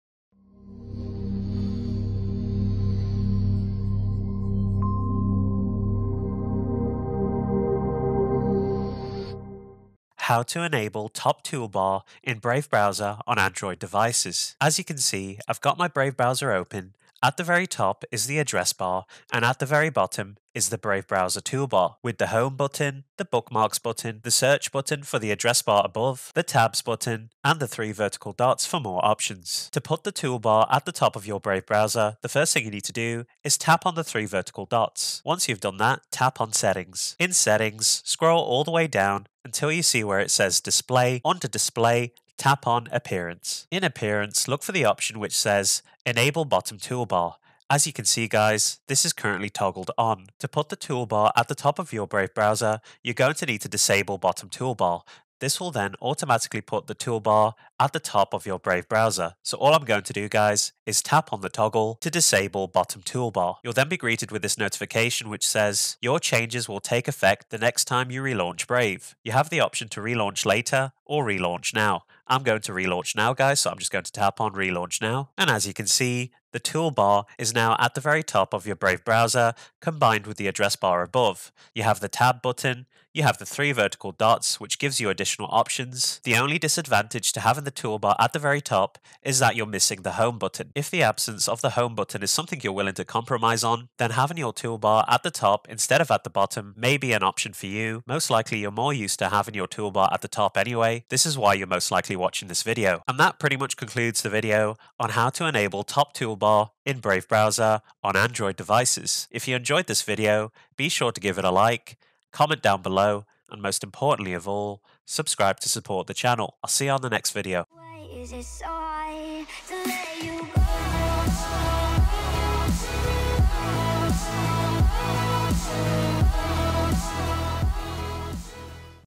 How To Enable Top Toolbar In Brave Browser On Android Devices
How To Move The Toolbar To The Top Of Brave Browser On Android Devices

In this video, I will be showing you how to move the Brave Browser toolbar from the bottom to the top of your browser. If you're coming from the Google Chrome browser or another browser you may be more used to having the toolbar at the top instead of the bottom of your browser. By default, Brave browser puts the toolbar at the bottom of your browser by enabling the appearance setting "enable bottom toolbar". This setting will need to be turned off or toggled off to move the toolbar to the top of your Brave browser on your Android device.

Steps To Move Brave Browsers Toolbar To The Top:
1. Open the Brave browser app
2. Tap on the three vertical dots at the bottom right hand corner
3. Tap on Settings
4. Tap on Appearance
5. Turn off or Toggle off "Enable bottom toolbar"

Congratulations, you have successfully moved the Brave browser toolbar from the bottom of the browser to the top.

Timestamps:
0:00 - Intro & Context
0:14 - Enable Top Toolbar In Brave Browser On Android
1:49 - Closing & Outro